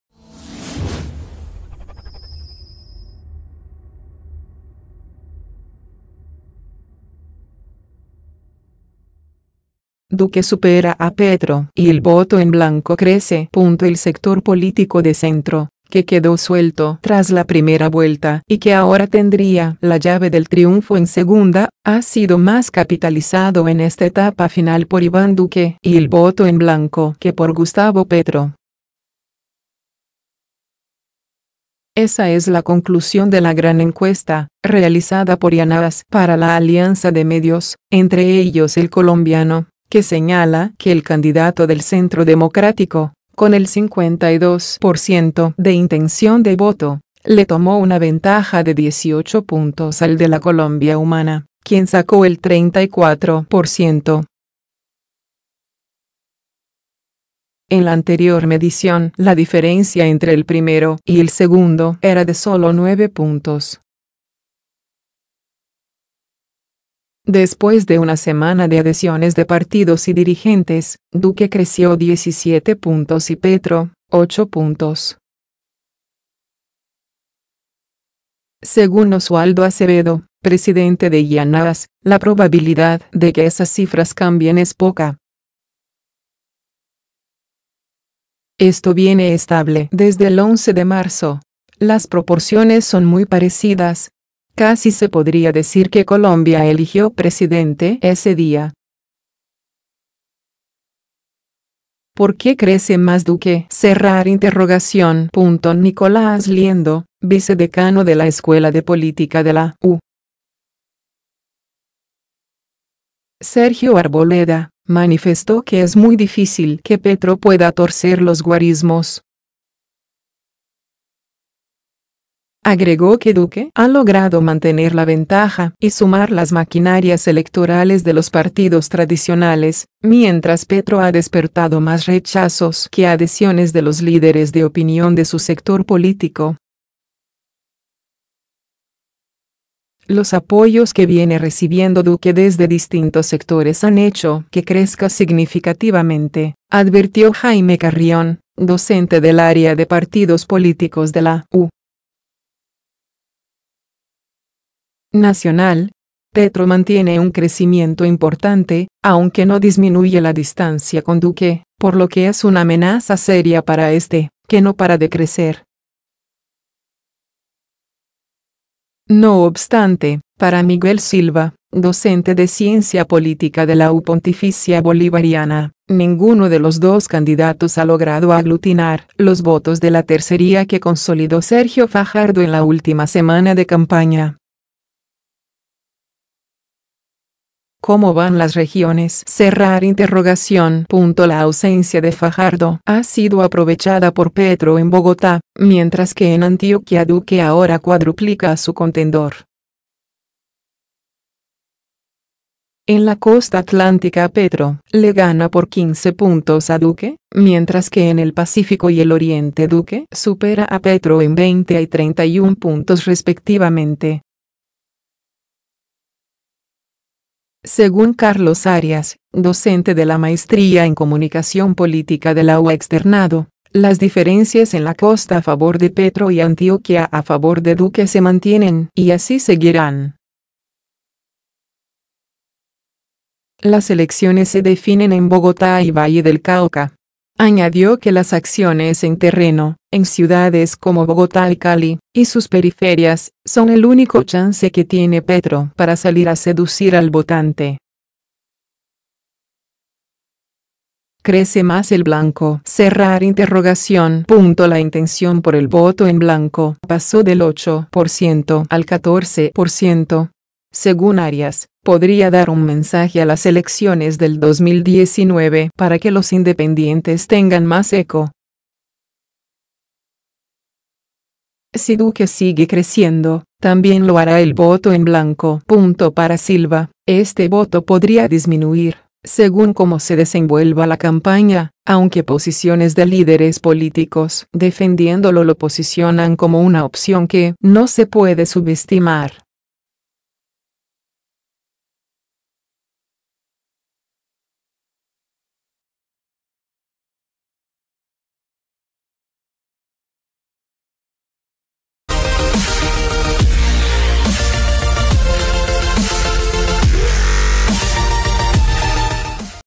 Elecciones 2018 : Duque supera a Petro y el voto en blanco crece
Duque supera a Petro y el voto en blanco crece.El sector político de centro, que quedó suelto tras la primera vuelta y que ahora tendría la llave del triunfo en segunda, ha sido más capitalizado en esta etapa final por Iván Duque y el voto en blanco que por Gustavo Petro. Esa es la conclusión de La Gran Encuesta, realizada por Yanhaas para la alianza de medios, entre ellos EL COLOMBIANO, que señala que el candidato del Centro Democrático, con el 52 % de intención de voto, le tomó una ventaja de 18 puntos al de la Colombia Humana, quien sacó el 34 %. En la anterior medición la diferencia entre el primero y el segundo era de solo 9 puntos.  Después de una semana de adhesiones de partidos y dirigentes, Duque creció 17 puntos y Petro, 8 puntos. Según Oswaldo Acevedo, presidente de YanHaas, la probabilidad de que esas cifras cambien es poca.  “Esto viene estable desde el 11 de marzo; las proporciones son muy parecidas. Casi se podría decirque Colombia eligió presidente ese día”. ¿Por qué crece más Duque?.Nicolás Liendo, vicedecano de la Escuela de Política de la U.  Sergio Arboleda, manifestó que es muy difícil que Petro pueda torcer los guarismos. Agregó que Duque ha logrado mantener la ventaja y sumar las maquinarias electorales de los partidos tradicionales, mientras Petro ha despertado más rechazos que adhesiones de los líderes de opinión de su sector político. Los apoyos que viene recibiendo Duque desde distintos sectores han hecho que crezca significativamente, advirtió Jaime Carrión, docente del área de Partidos Políticos de la U.  Nacional. “Petro mantiene un crecimiento importante, aunque no disminuye la distancia con Duque, por lo que es una amenaza seria para este, que no para de crecer”. No obstante, para Miguel Silva, docente de Ciencia Política de la U. Pontificia Bolivariana, ninguno de los dos candidatos ha logrado aglutinar los votos de la tercería que consolidó Sergio Fajardo en la última semana de campaña. ¿Cómo van las regiones?.La ausencia de Fajardo ha sido aprovechada por Petro en Bogotá, mientras que en Antioquia Duque ahora cuadruplica a su contendor.  En la Costa Atlántica Petro le gana por 15 puntos a Duque, mientras que en el Pacífico y el Oriente Duque supera a Petro en 20 y 31 puntos respectivamente. Según Carlos Arias, docente de la Maestría en Comunicación Política de la U. Externado, las diferencias en la costa a favor de Petro y Antioquia a favor de Duque se mantienen y así seguirán.  “Las elecciones se definen en Bogotá y Valle del Cauca”. Añadió que las acciones en terreno, en ciudades como Bogotá y Cali, y sus periferias, son el único chance que tiene Petro para salir a seducir al votante. ¿Crece más el blanco?.La intención por el voto en blanco pasó del 8 % al 14 %. Según Arias, podría dar un mensaje a las elecciones del 2019 para que los independientes tengan más eco.  “Si Duque sigue creciendo, también lo hará el voto en blanco”.Para Silva, este voto podría disminuir, según como se desenvuelva la campaña, aunque